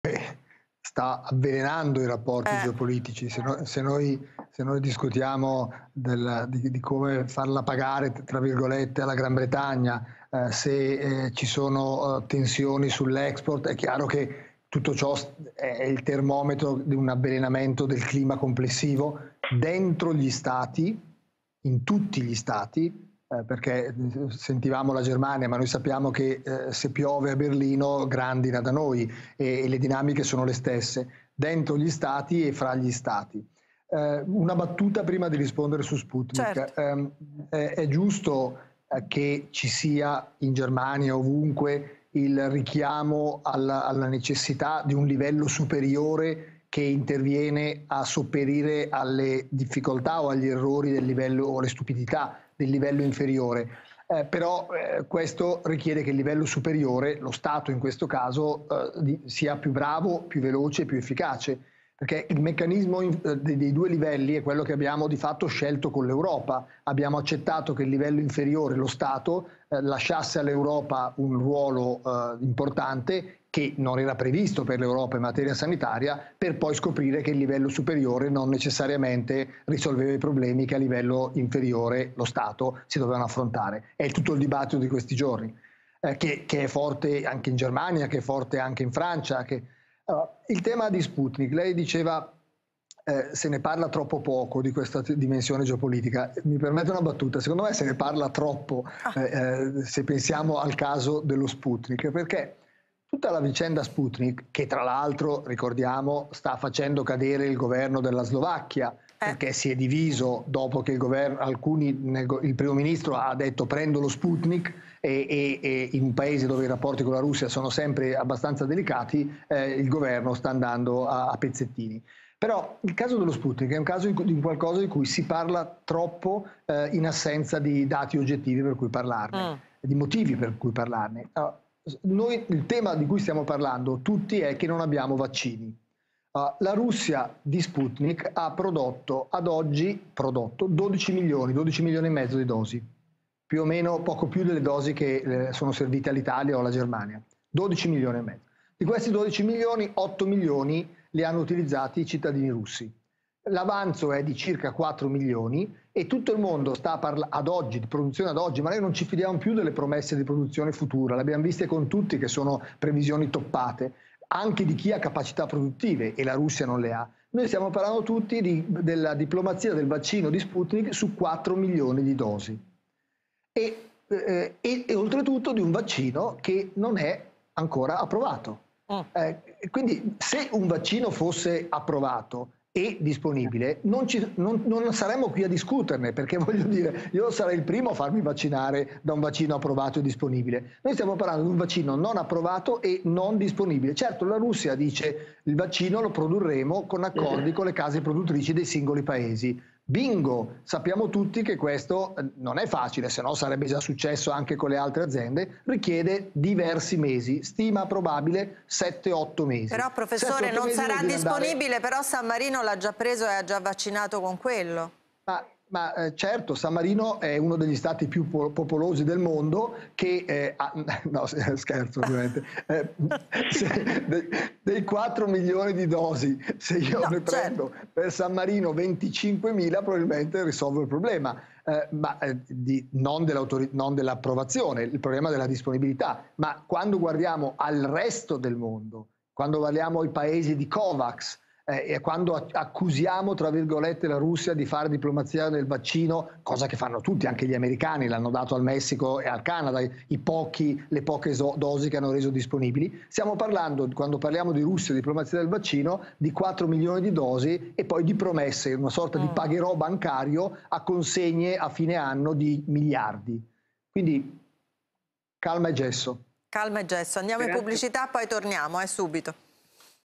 Vaccini, Paolo Magri: "Si sta avvelenando il clima geopolitico dentro gli Stati e fra gli ...
Paolo Magri (ISPI) sulla geopolitica dei vaccini: "Si sta avvelenando il clima sia dentro gli Stati sia tra gli Stati. Sputnik? La Russia ha prodotto solo 12 milioni di dosi e utilizzate 8..."